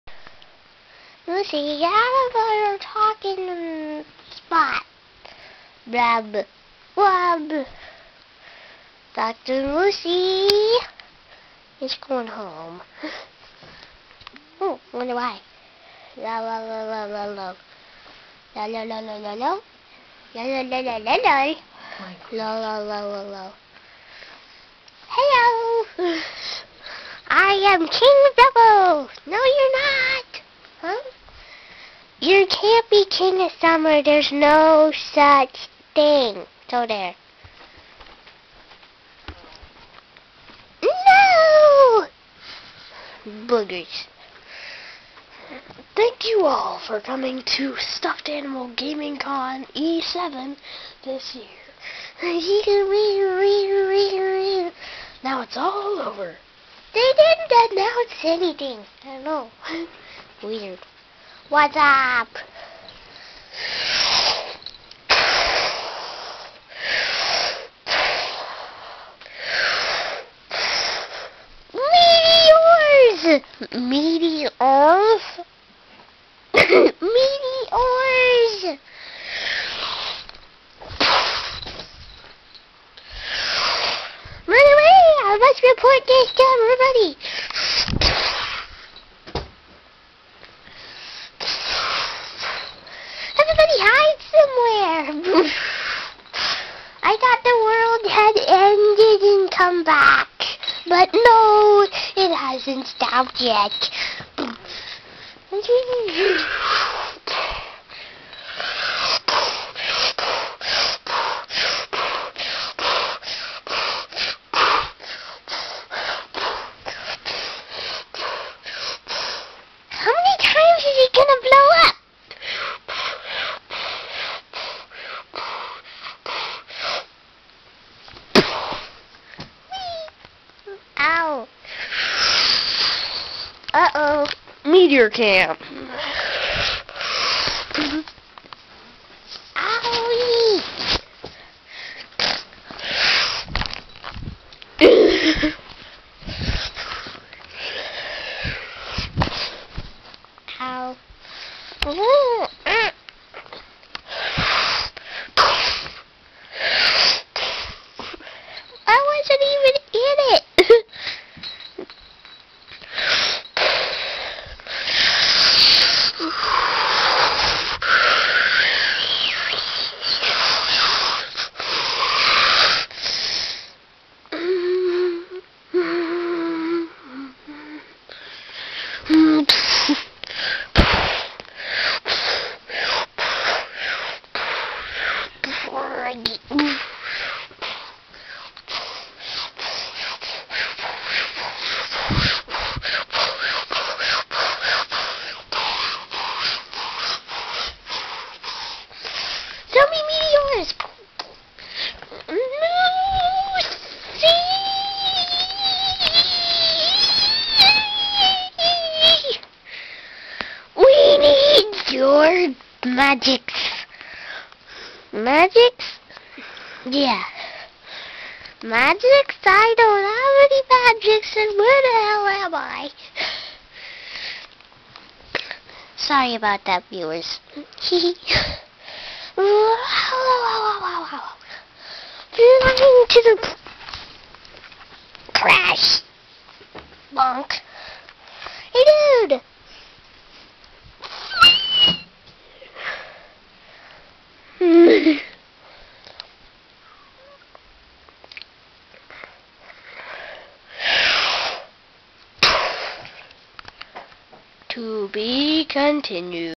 Stuft Animals Episode 46 (Meaty-Ores, Part 1)
The season 2 Finale's first part is here! w00t! Meaty-Ores attack the stufts. What will happen? Who knows?